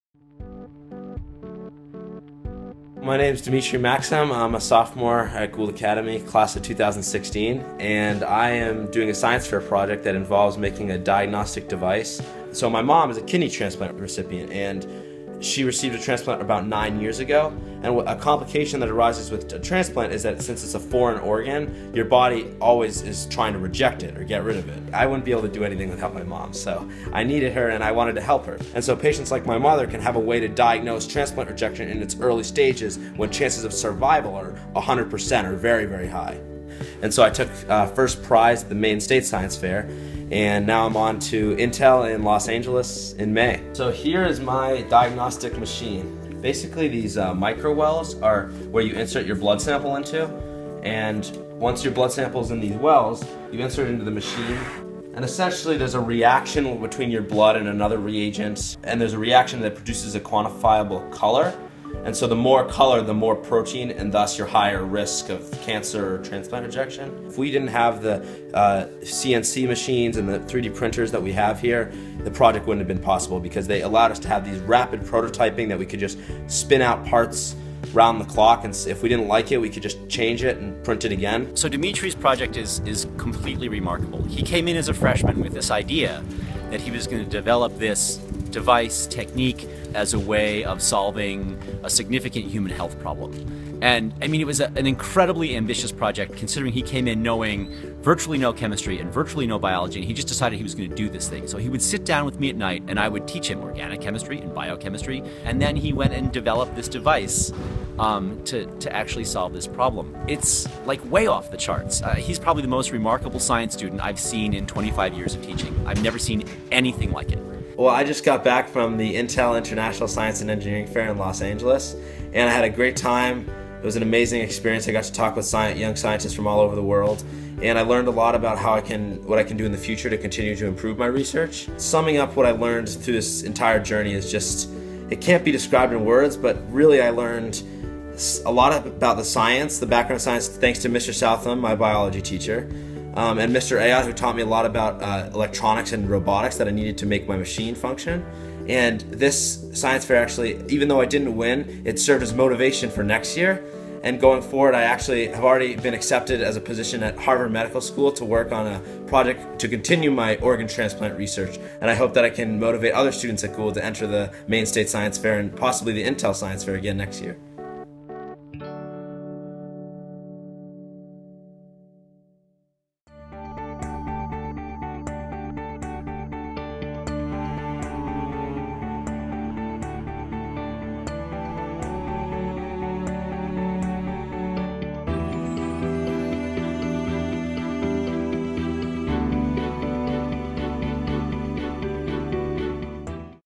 Gould goes to The Intel ISEF
Demetri is a remarkable student. During his Sophomore year at Gould he created a medical device to detect kidney rejection following transplantation. Demetri's device can also be used to detect ovarian cancer. His device picks up these diseases in their early stages, when patients have close to a 100% chance of survival.

After winning the Maine State Science Fair in March he went on to compete at the Intel International Science and Engineering Fair (ISEF) in Los Angeles, California this May.